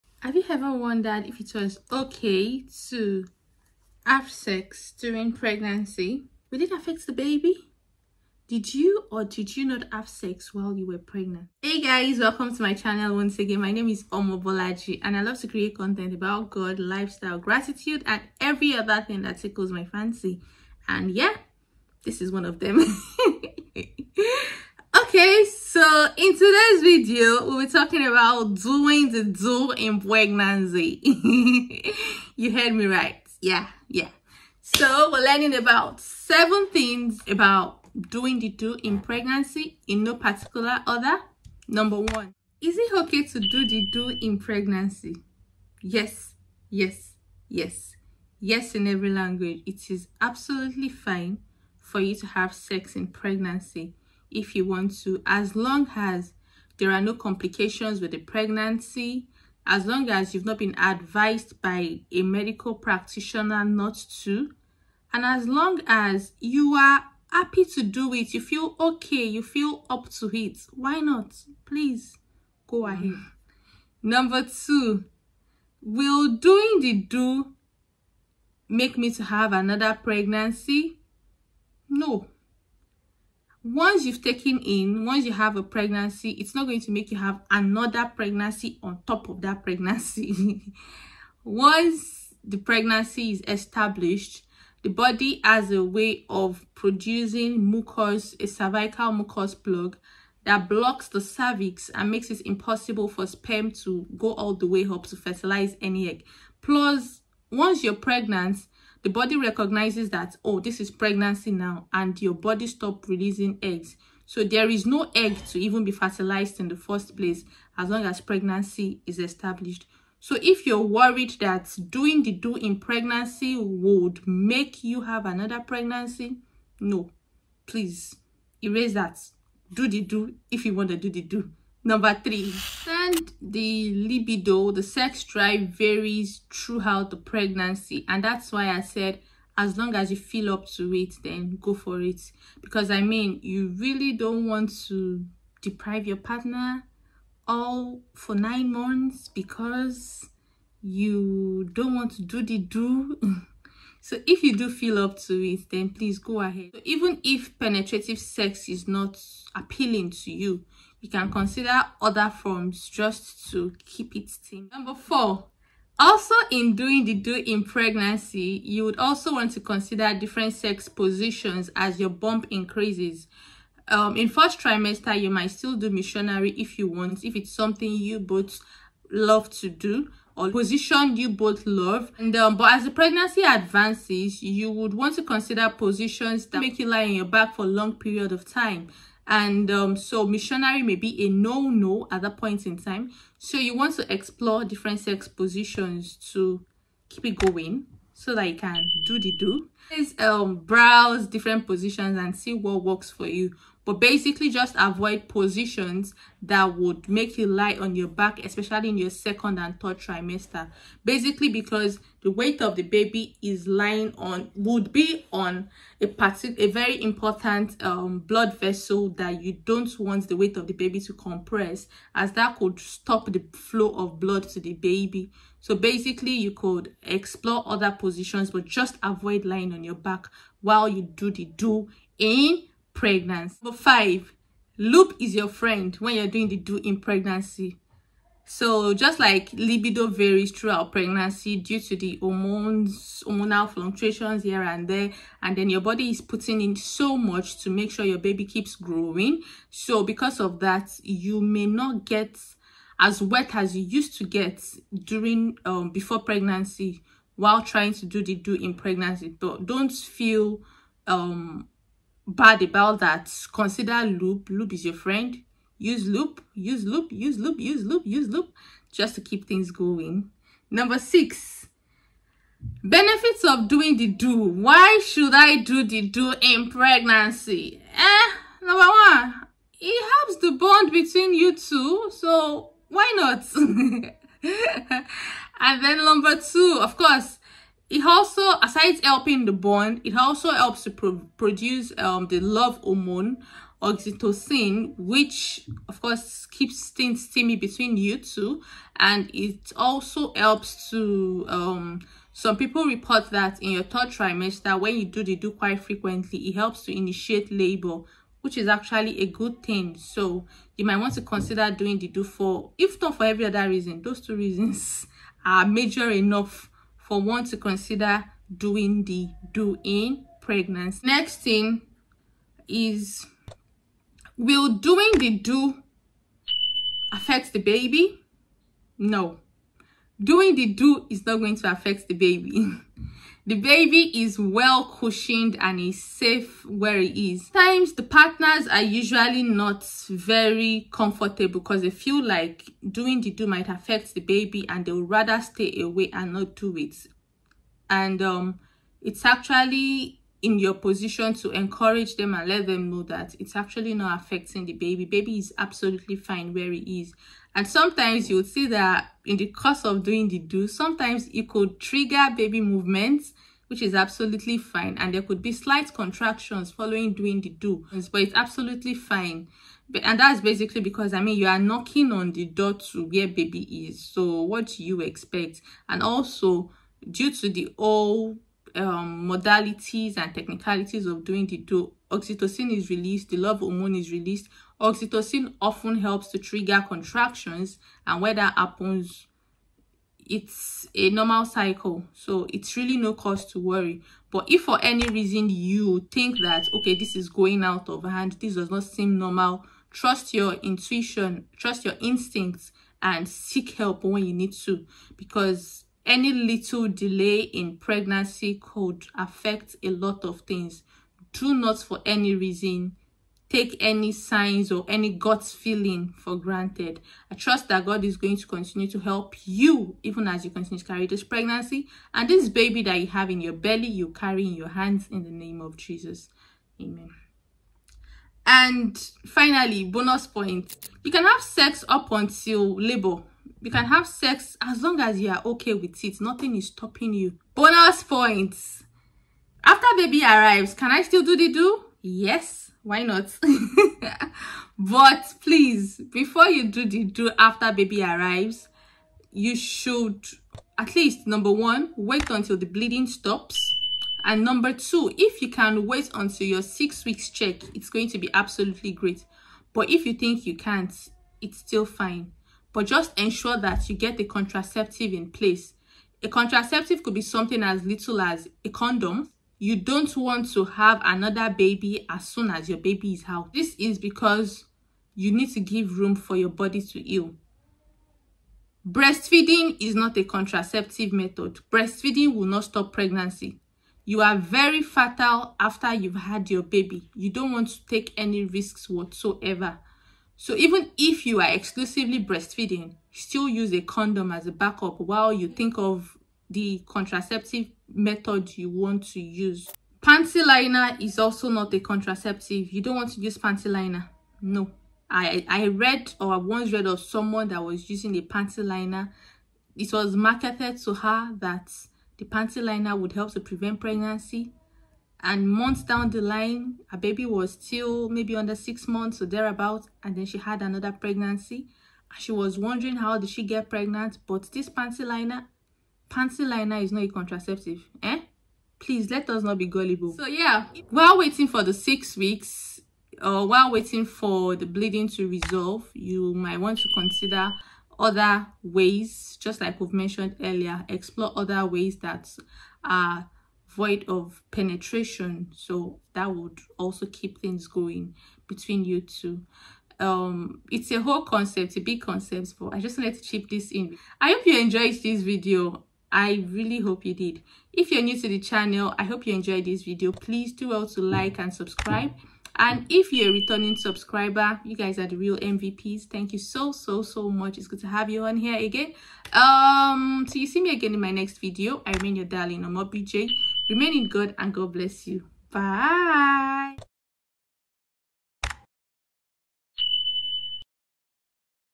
Is it ok to have sex in pregnancy?Does it affect baby & more
This video highlights a couple of points regarding sex in pregnancy. Enjoy 🤗

Let’s connect;